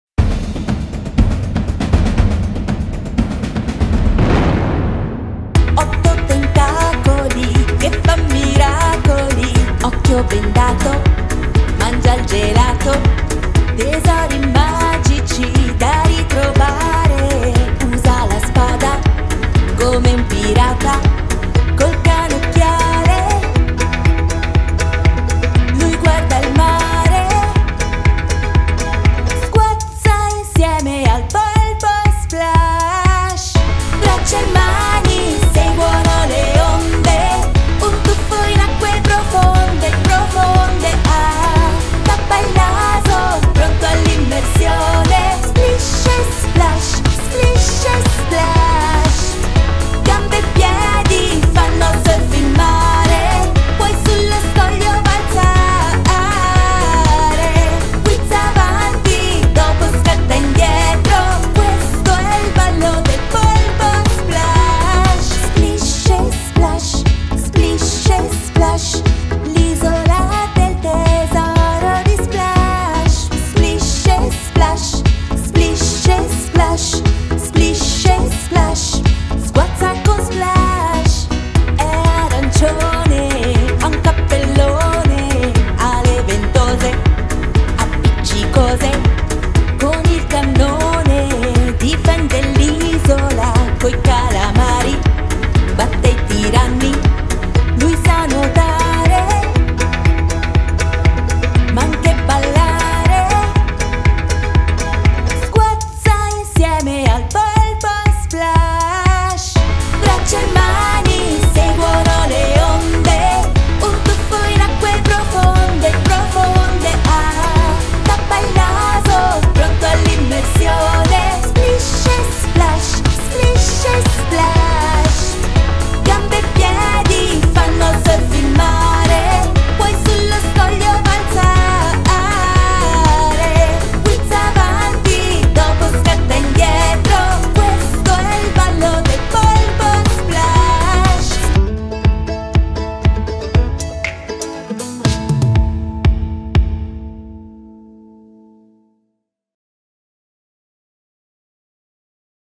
PARCO LA QUIETE - Vichinghi Day - 22 Agosto 2021
VICHINGHI DAY - JUST YOUR PLACE

Venite a scoprire il Magico Mondo del Parco La Quiete, Con i nostri Amici Stewie La Talpa, Olly Il Drago, Splash il Polpo e con Capitan Danny Barba Nera e tutti i i personaggi Fantastici.